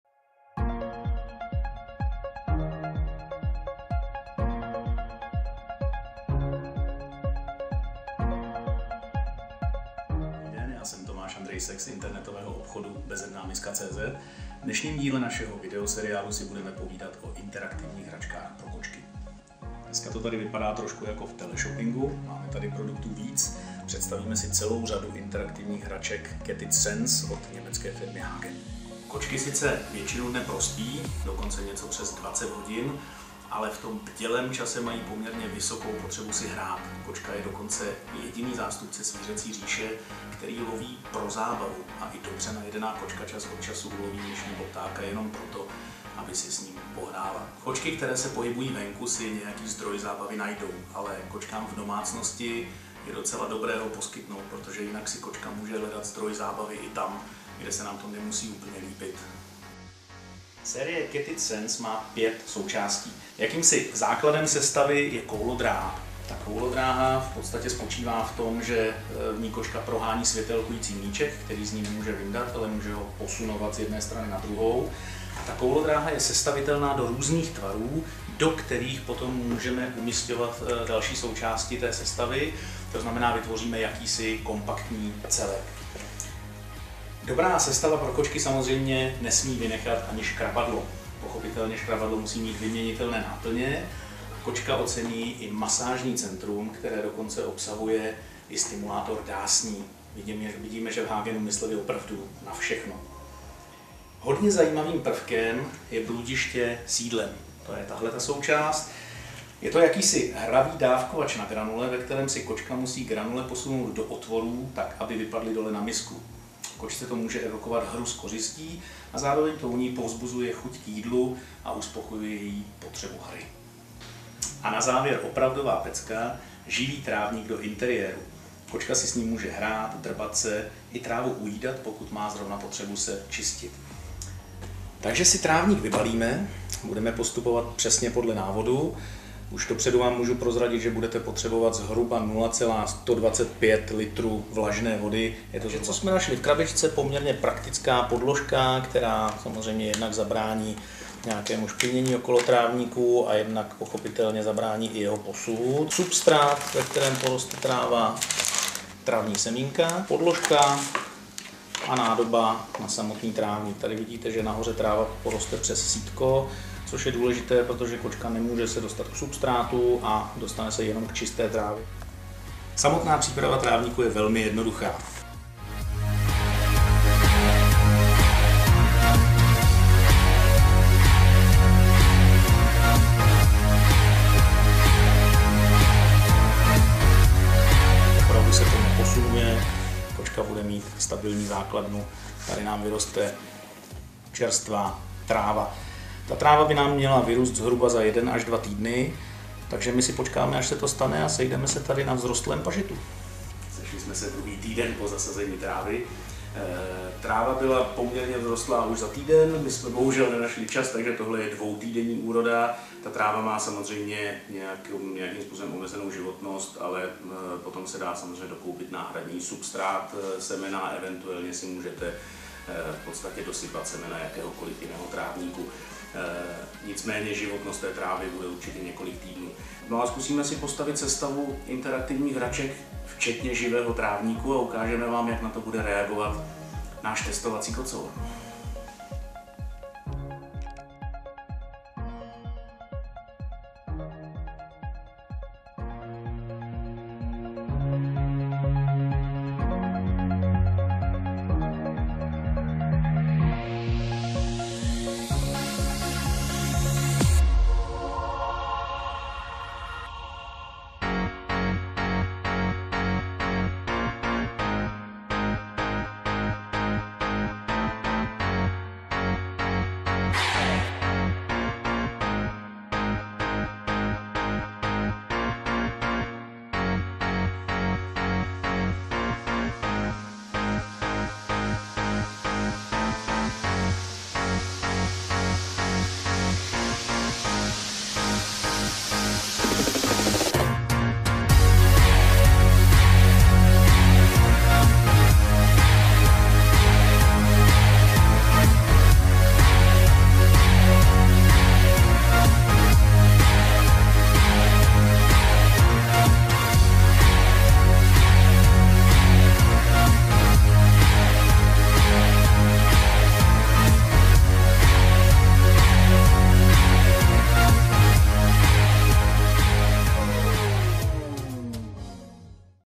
CAT IT Design Senses - Interaktivní hračky pro kočky
Bezedná miska TV představuje krátké video o interaktivních hračkách pro kočky CAT IT Design Senses v čele s Tomášem Andrejskem :)
Pokud máte zájem a chcete si zjistit více, naleznete to zde :